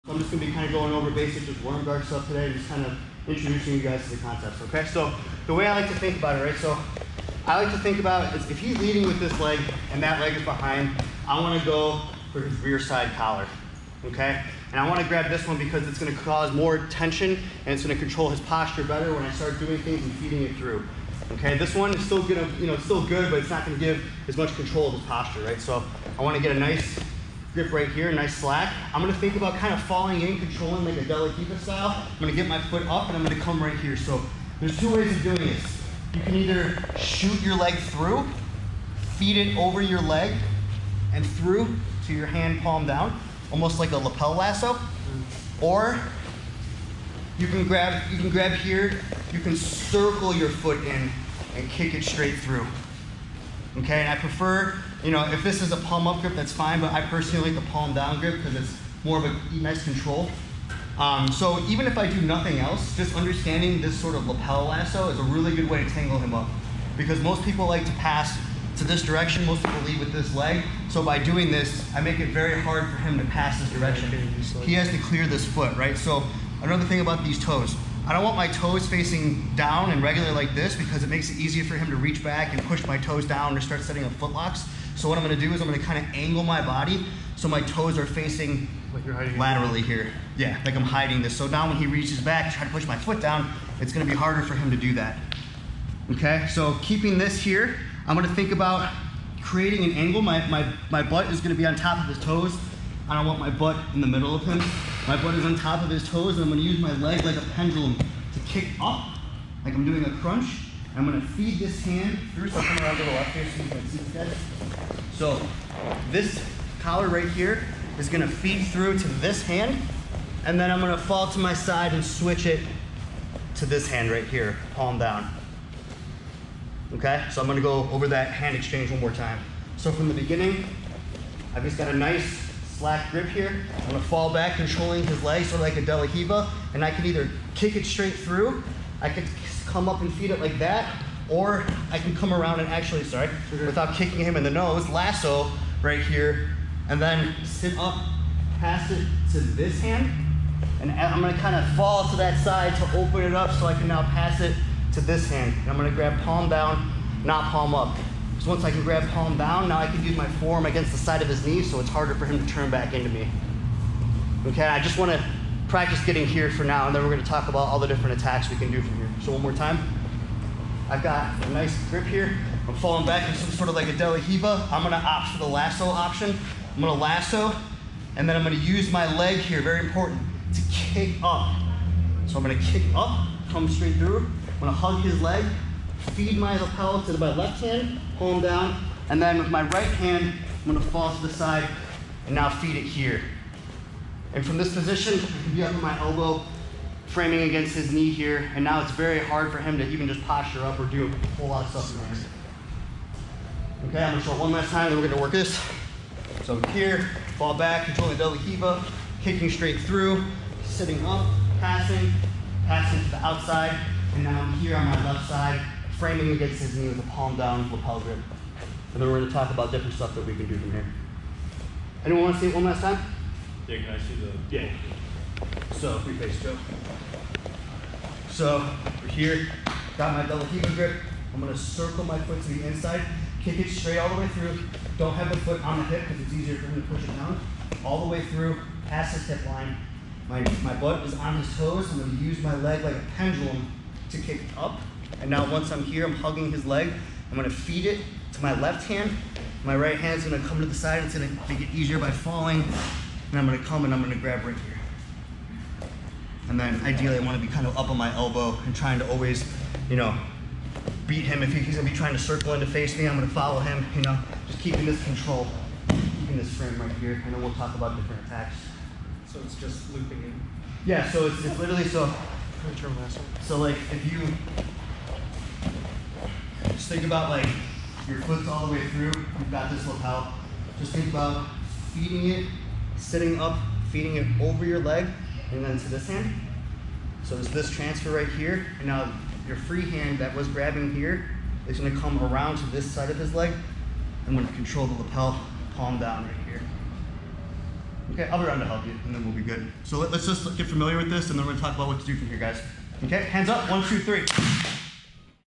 Worm Guard Setup and Control
professor Blake Klassman shows the very crucial first steps to setting up and maintaining the worm guard.